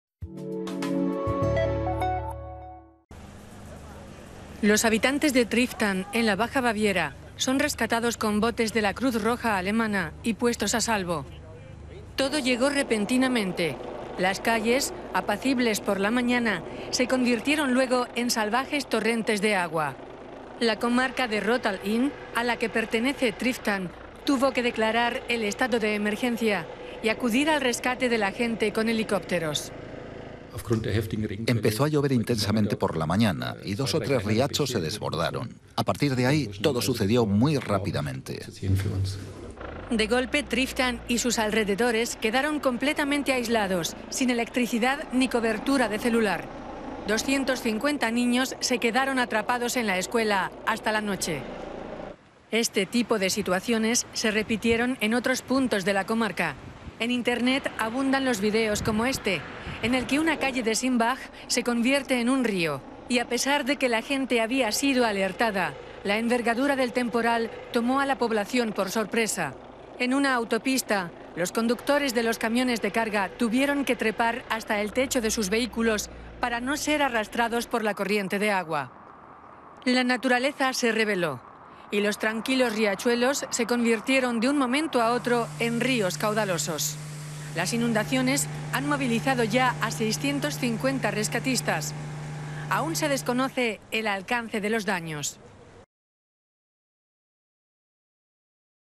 Inundaciones en Baja Baviera
Las lluvias torrenciales convirtieron los apacibles arroyos en acaudalados ríos en Triftern, Baviera.